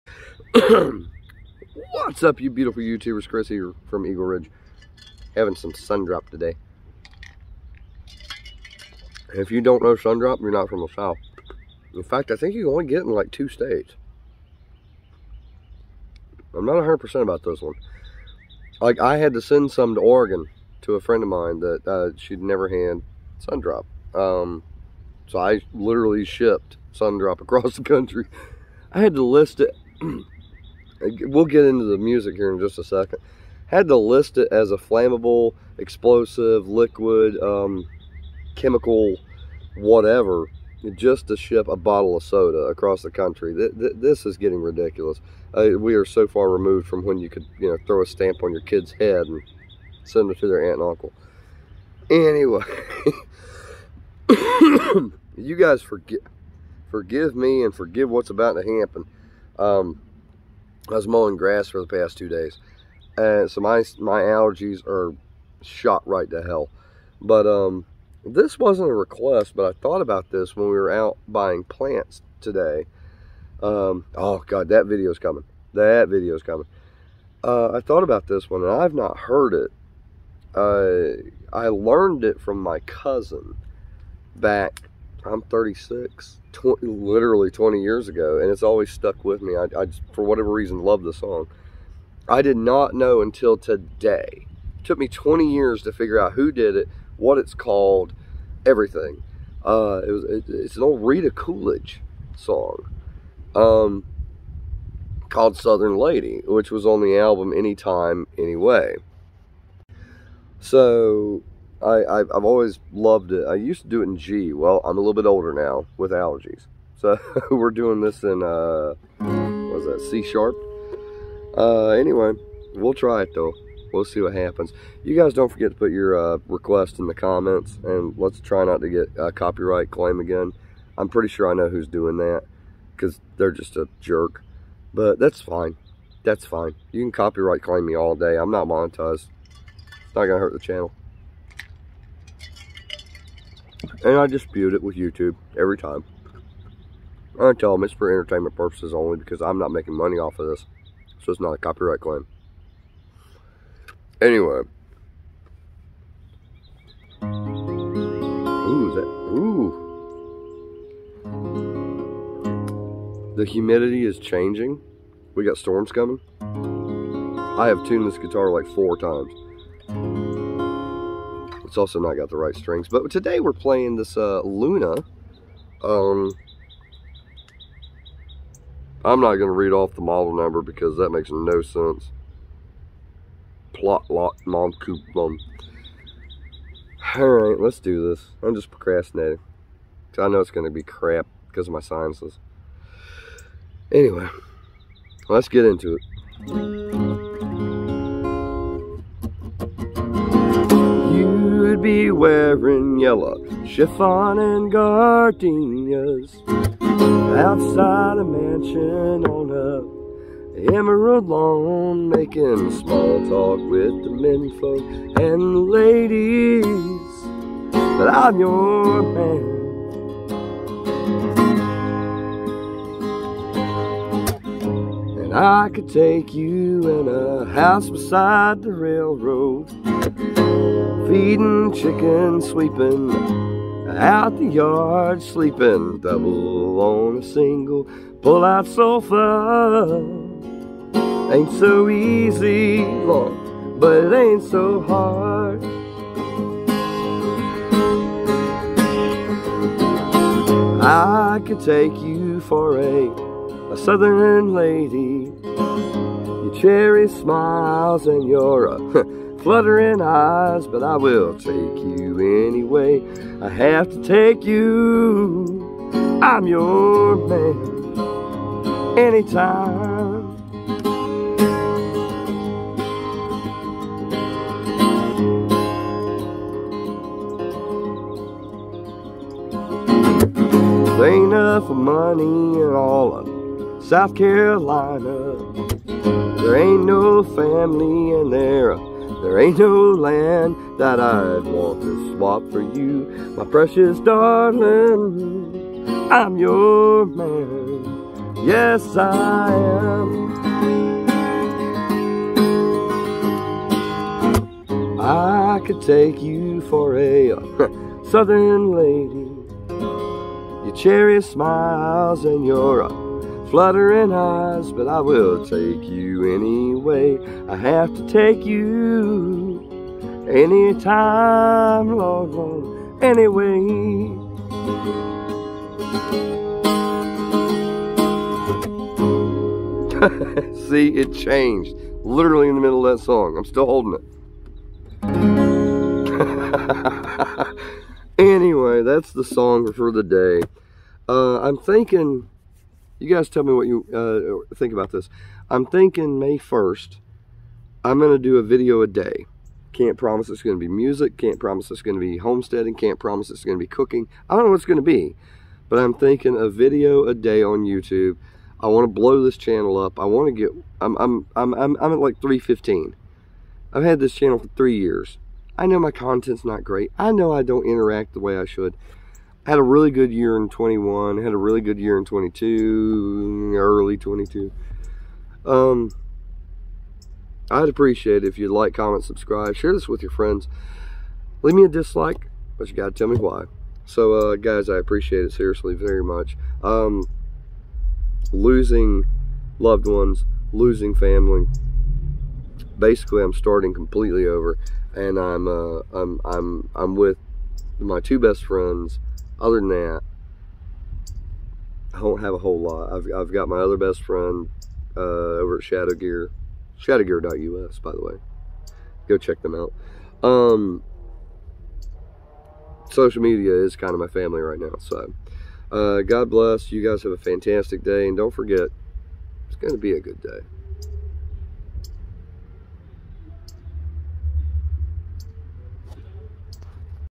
New Old Music!!!!
New music I learned 20 years ago! You guys hang around for the announcement at the end of the video!

EAGLE RIDGE SOCIAL MEDIA LINKS